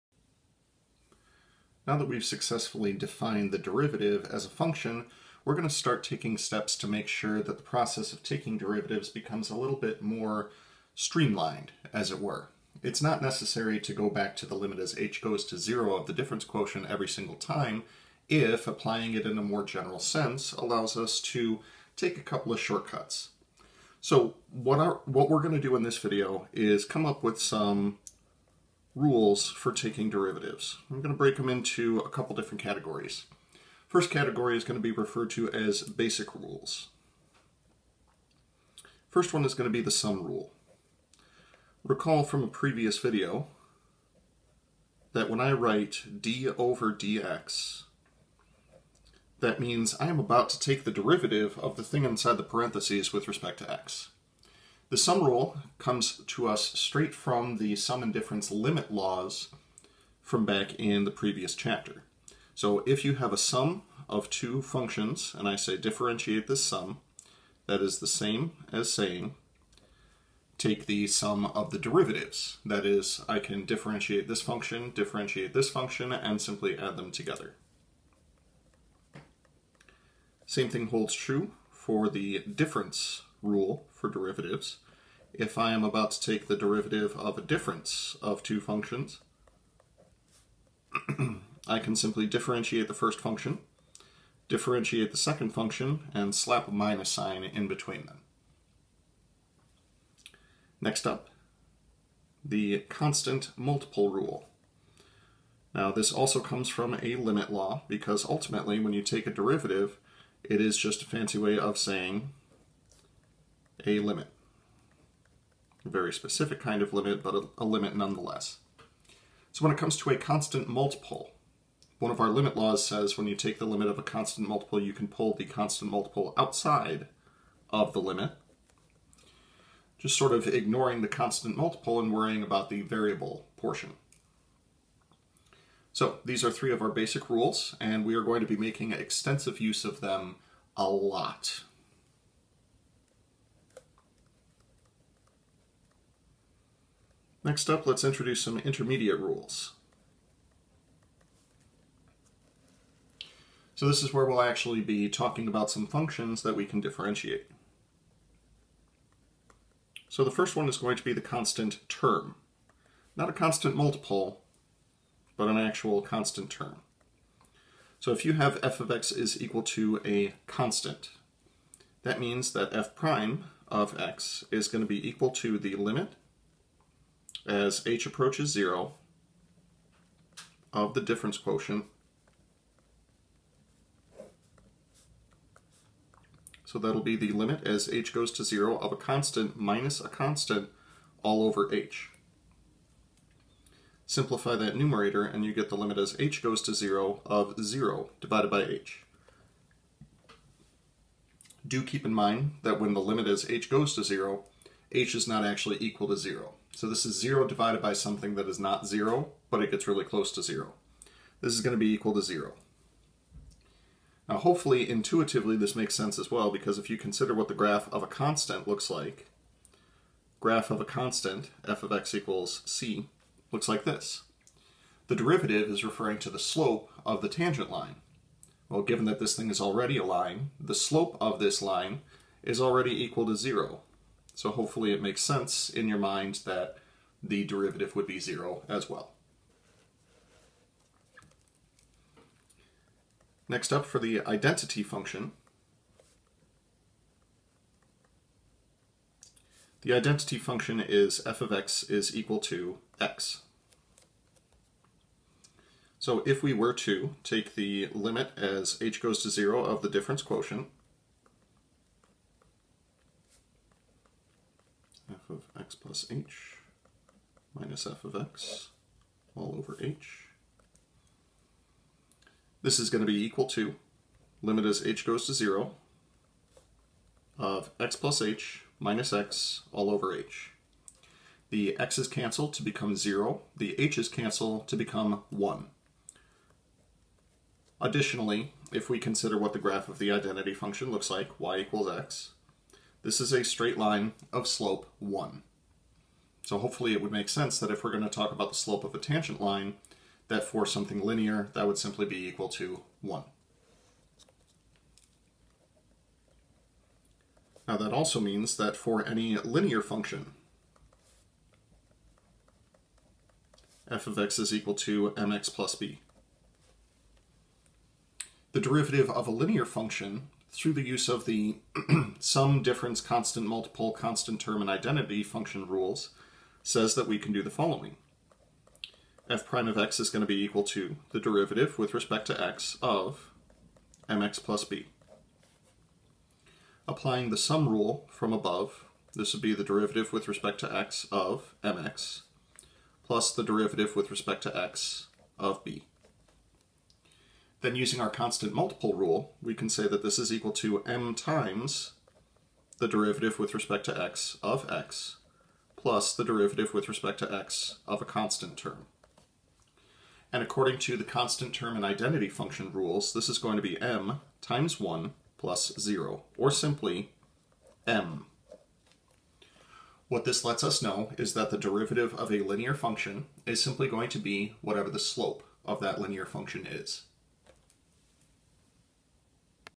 Derivative shortcuts: the basics
These are the rules for derivatives that get used really frequently. They will become second nature in no time.